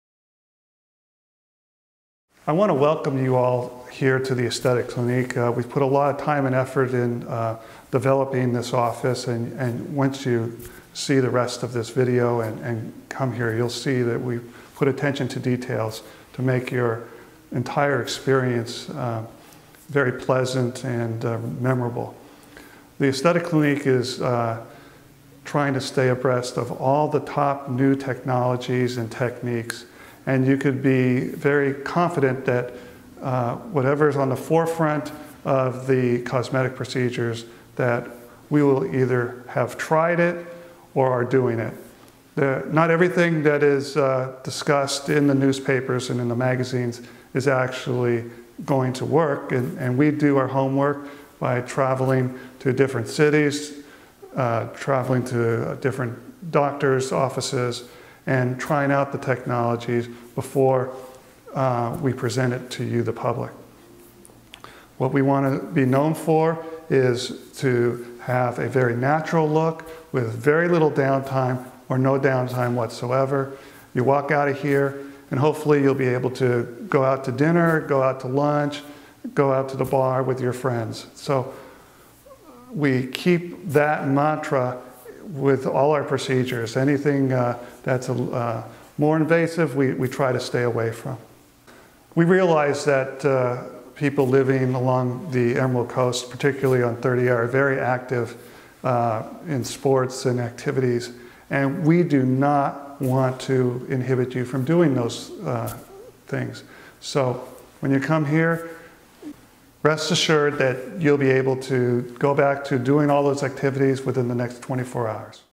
The Aesthestic Clinique's Philosophy Minimal Invasive Procedures, Steve Weiner, MD
The Aesthetic Clinique was opened in 2005 by Dr. Steve Weiner, Facial Plastic Surgeon.  He recognized that the trend of cosmetic procedures was towards non-invasive or minimally invasive with little to no downtime.  He laid down his scalpel and now devotes 100% of his time to Lasers, Injectables, and non invasive skin tightening and body contouring procedures.